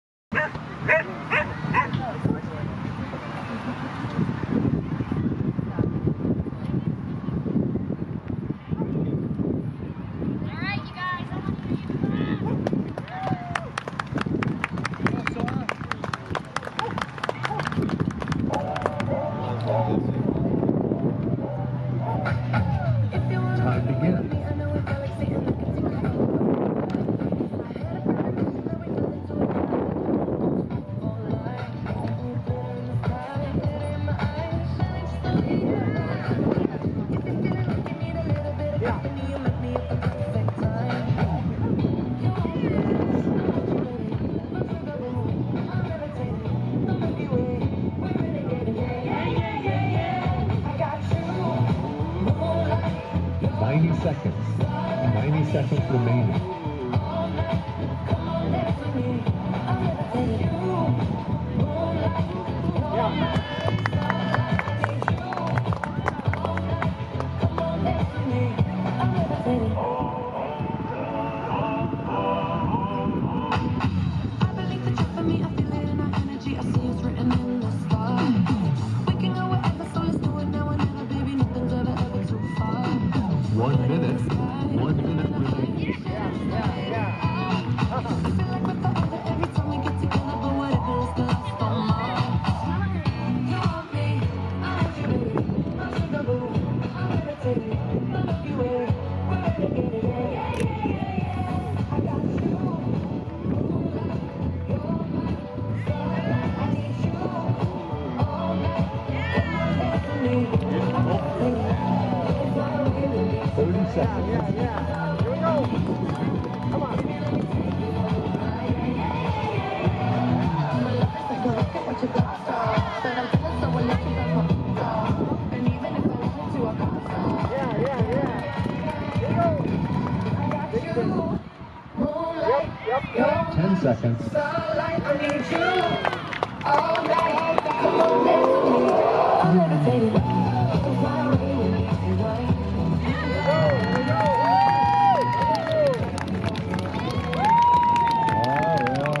Kirby & Sora Round 2 ~Ashley Whippet Invitational World Finals Open Freestyle
Second round, first place

All music rights are reserved to the owner of the copyright

Naperville, Illinois, 2024